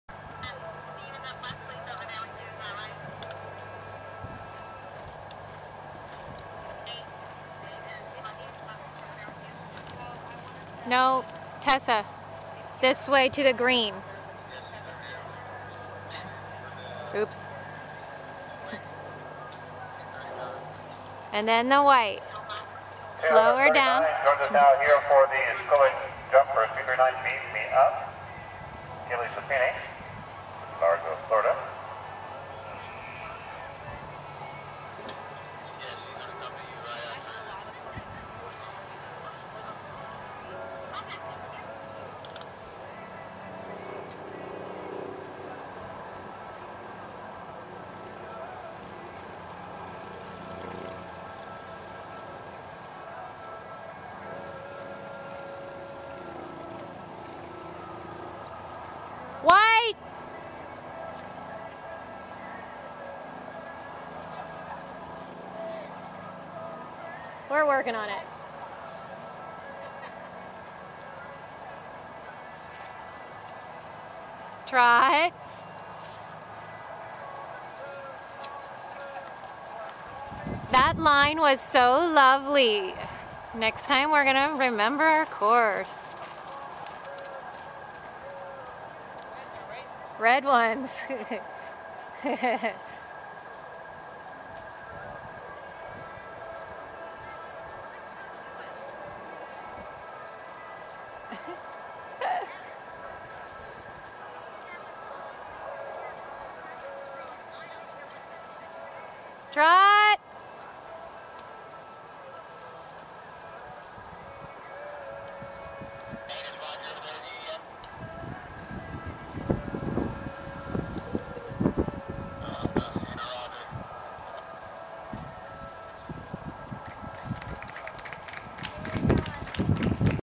Laurel Oaks Stables Peaches and Tess- Wrong way course!
Fox Lea Farms, 10-14-12
Tess and Peaches first "big girl" course, along with a little glitch!  lol
You'll get it next time!